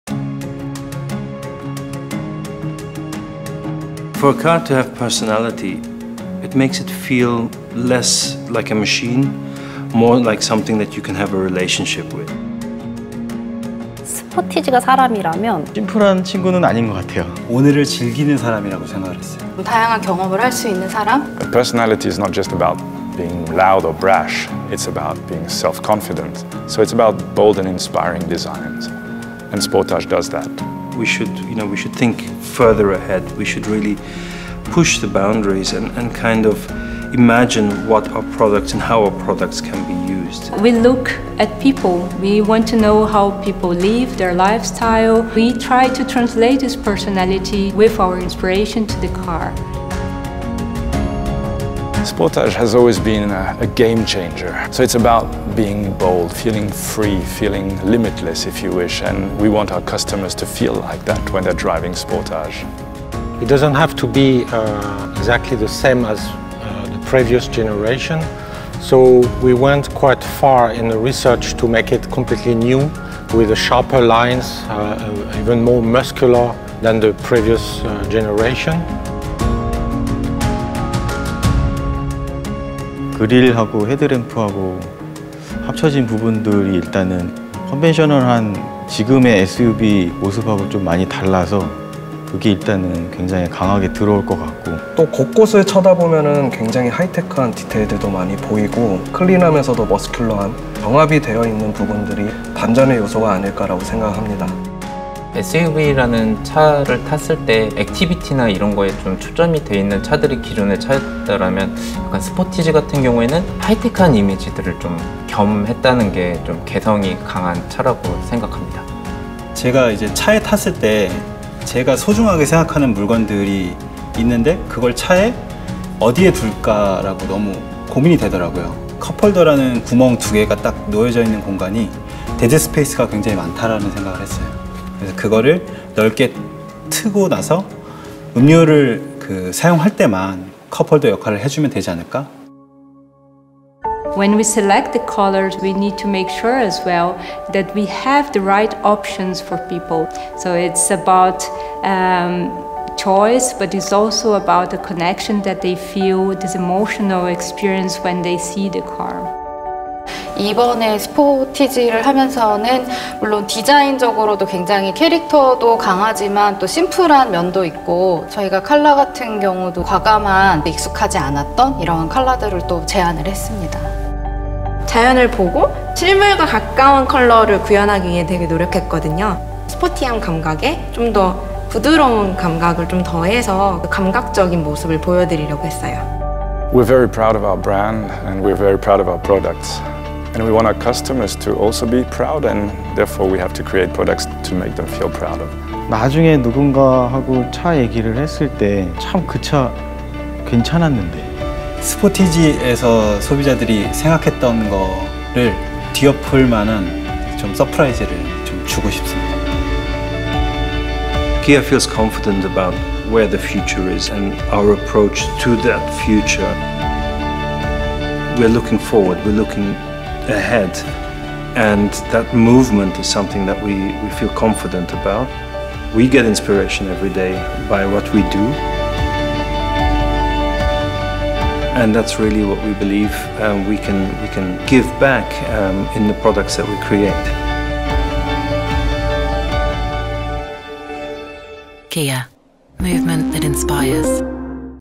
The Kia Sportage : Source of Inspiration l Part 1. Design
The all-new Sportage. Embodying both bold and inspiring aspects of design while its sophisticated interior holds a multitude of advanced technological features.

Discover what inspired us to create the all-new Sportage.